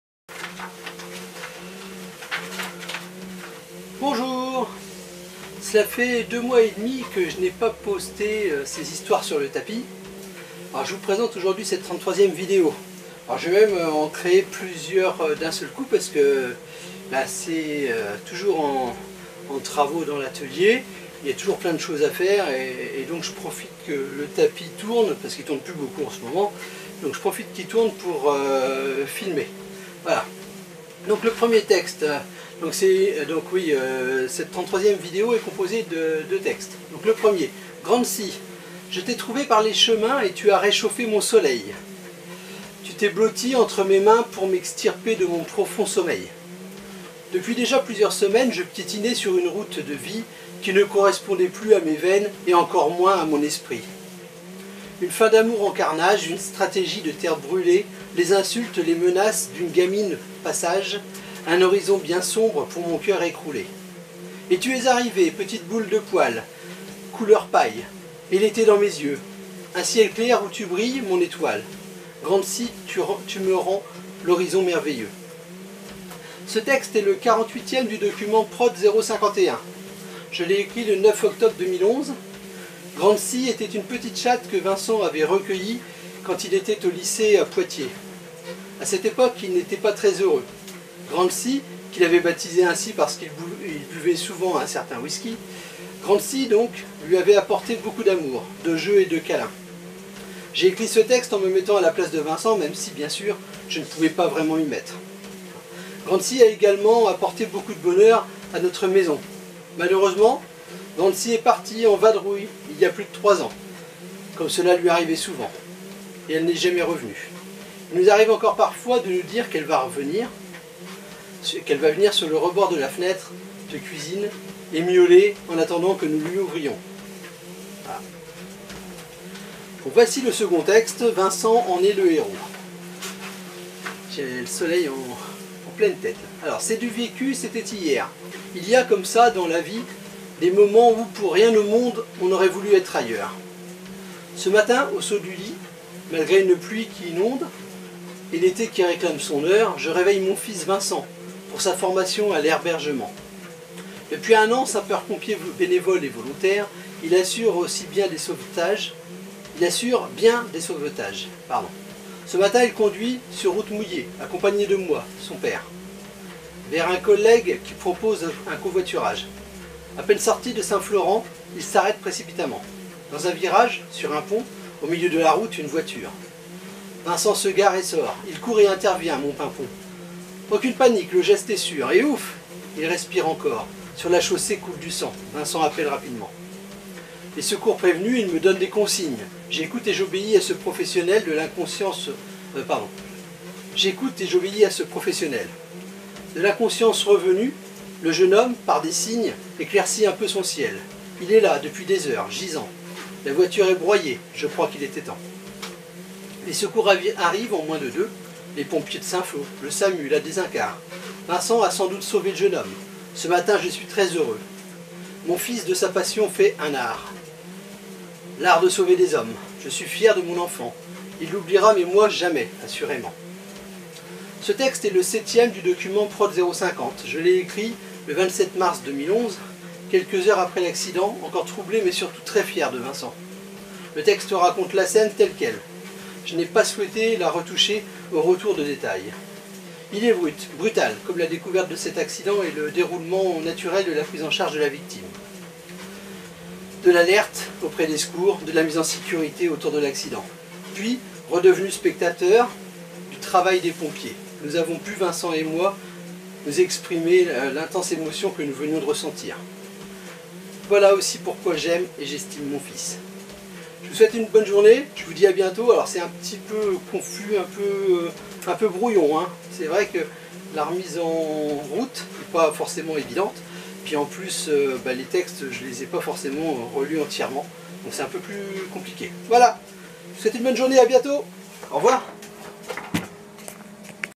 Amajyp met ses histoires sur le tapis 33/40 et plus
Je vous présente aujourd’hui cette 33ème vidéo.

Elle est composée de 2 textes.

GRANTSY

Je t’ai trouvée par les chemins
Et tu as réchauffé mon soleil.
Tu t’es blottie entre mes mains
Pour m’extirper de mon profond sommeil.

Depuis déjà plusieurs semaines
Je piétinais sur une route de vie
Qui ne correspondait plus à mes veines
Et encore moins à mon esprit.
Une fin d’amour en carnage ;
Une stratégie de terre brûlée.
Les insultes, les menaces d’une gamine pas sage.
Un horizon bien sombre pour mon cœur écroulé.

Et tu es arrivée, petite boule de poils
Couleur paille. Et l’été dans mes yeux.
Un ciel clair où tu brilles, mon étoile.
Grantsy, tu me rends l’horizon merveilleux.

Ce texte est le 48ème du document PROT051. Je l’ai écrit le 09 octobre 2011. Grantzy était une petite chatte que Vincent avait recueillie quand il était au Lycée à Poitiers. A cette époque, il n’était pas très heureux. Grantzy, qu’il avait baptisée ainsi parce qu’il buvait souvent un certain whisky… Grantzy lui avait apporté beaucoup d’amour, de jeux et de câlins.

J’ai écrit ce texte en me mettant à la place de Vincent même si, bien sûr, je ne pouvais pas vraiment m’y mettre.

Grantzy a également apporté beaucoup de bonheur à notre maison. Malheureusement, Grantzy est partie en vadrouille il y a plus de trois ans, comme cela lui arrivait souvent… et elle n’est jamais revenue.

Il nous arrive encore parfois de nous dire qu’elle va venir sur le rebord de la fenêtre de cuisine et miauler en attendant que nous lui ouvrions.

Le second texte est
C’EST DU VECU, C’ETAIT HIER

Je vous laisse le découvrir sur la vidéo.
Bonne fin de journée et à bientôt,